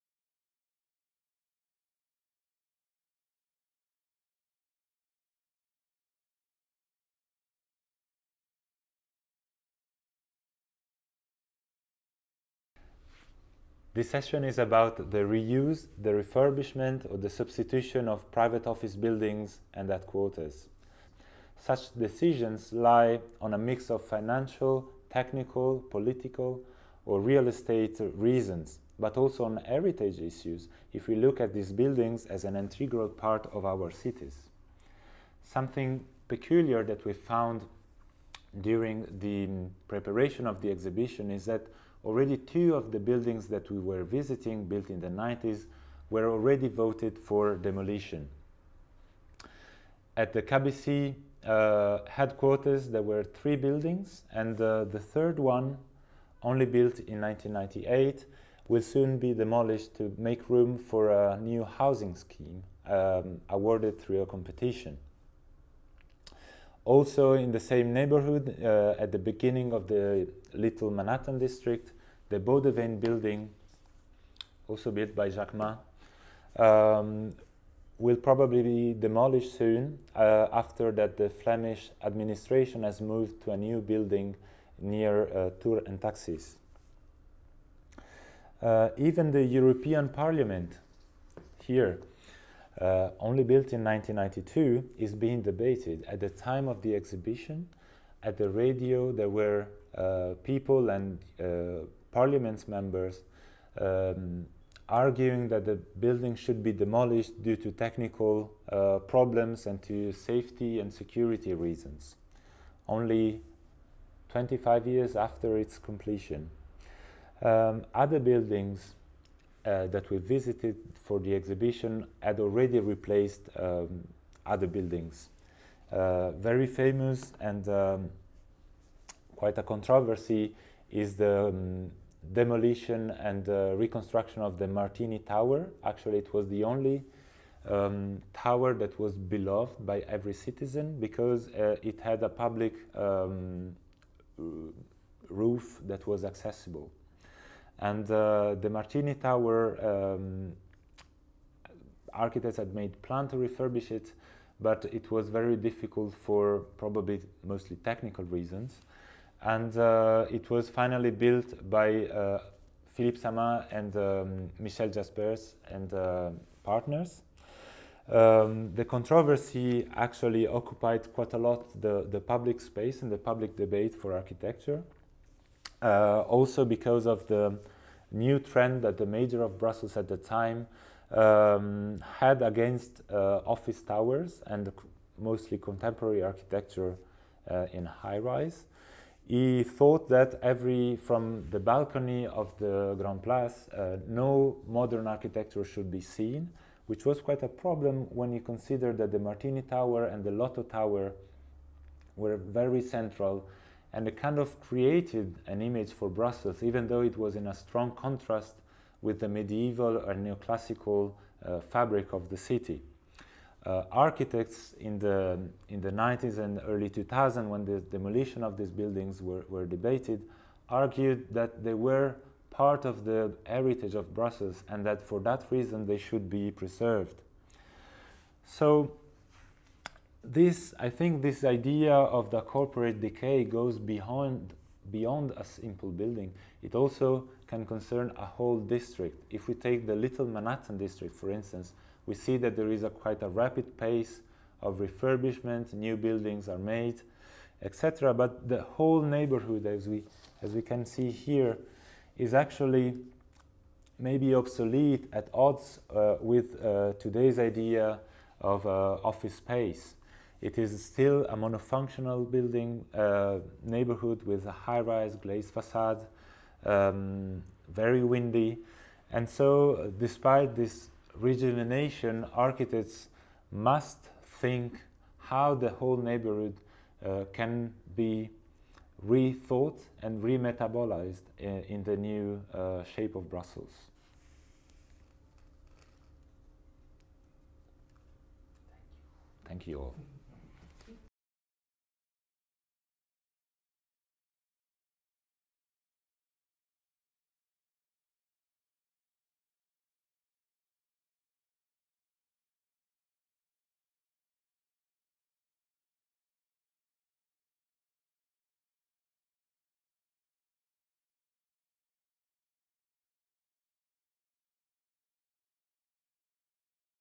Corporate Arcadia TU Delft 1 Corporate Decay
Corporate Arcadia looks at what is usually disregarded by architecture culture: the corporate architecture of banking headquarters, office buildings and hotels that has mostly shaped the city of Brussels in the 1990s and early 2000s. This short movie is an addition on the Corporate Arcadia exhibition.